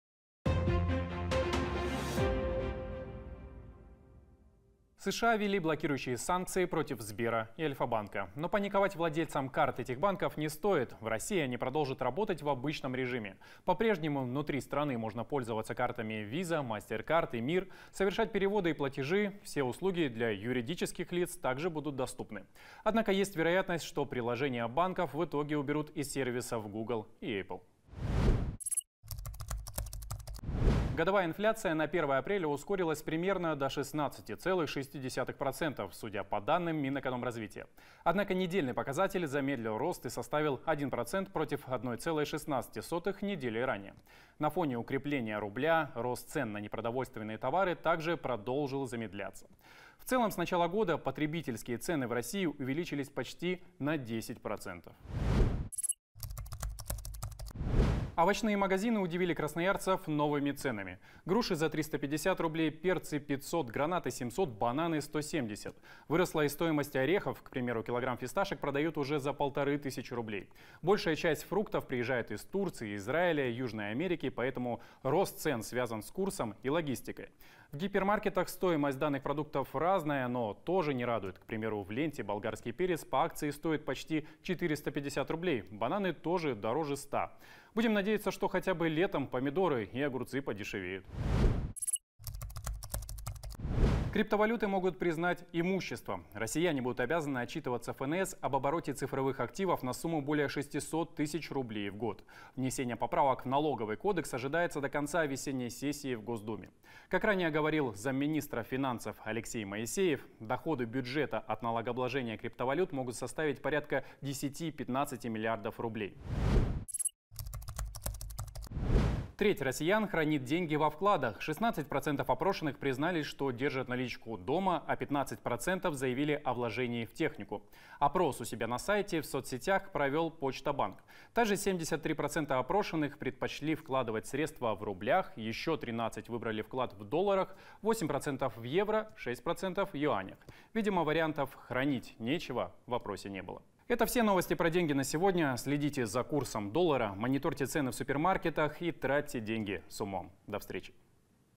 «Новости про деньги»: инфляция, овощи и крипта
США ввели блокирующие санкции против «Сбера» и «Альфабанка». Но паниковать владельцам карт этих банков не стоит, в России они продолжат работать в обычном режиме. По-прежнему внутри страны можно пользоваться картами Visa, Mastercard и «Мир», совершать переводы и платежи, все услуги для юридических лиц также будут доступны. Однако есть вероятность, что приложения банков в итоге уберут и с сервисов Google и Apple

Годовая инфляция на 1 апреля ускорилась примерно до 16,6%, судя по данным Минэкономразвития. Однако недельный показатель замедлил рост и составил 1% против 1,16 сотых неделей ранее. На фоне укрепления рубля рост цен на непродовольственные товары так же продолжил замедляться. В целом с начала года потребительские цены в России увеличились почти на 10%.

Овощные магазины удивили Красноярцев новыми ценами. Груши за 350 рублей, перцы — 500, гранаты — 700, бананы — 170. Выросла и стоимость орехов, к примеру, кг фисташек продают уже за 1500 рублей. Большая часть фруктов приезжает из Турции, Израиля, Южной Америки, поэтому рост цен связан с курсом и логистикой. В гипермаркетах стоимость данных продуктов разная, но тоже не радует, к примеру в «Ленте» болгарский перец по акции стоит почти 450 рублей, бананы дороже ста. Будем надеется, что хотя бы летом помидоры и огурцы подешевеют.

Криптовалюты могут признать имуществом, россияне будут обязаны отчитываться в ФНС об обороте цифровых активов на сумму более 600 тысяч рублей в год. Внесение поправок в налоговый кодекс ожидается до конца весенней сессии в Госудуме. Как ранее говорил замминистра финансов Алексей Моисеев, доходы бюджета от налогообложения криптовалют могут составить порядка 10–15 миллиардов рублей.

Треть россиян хранит деньги во вкладах, 16% опрошенных признались, что держат наличку дома. А 15% заявили о вложениях в технику. Так же 73% опрошенных предпочли вкладывать средства в рубли. Еще 13% выбрали вклад в долларах, 8% — в евро, 6% — в юанях. Видимо варианта — хранить нечего, в опросе не было.

Это все новости про деньги на сегодня, следите за курсом доллара, мониторьте цены в супермаркетах и тратьте деньги с умом. До встречи.«Новости про деньги»: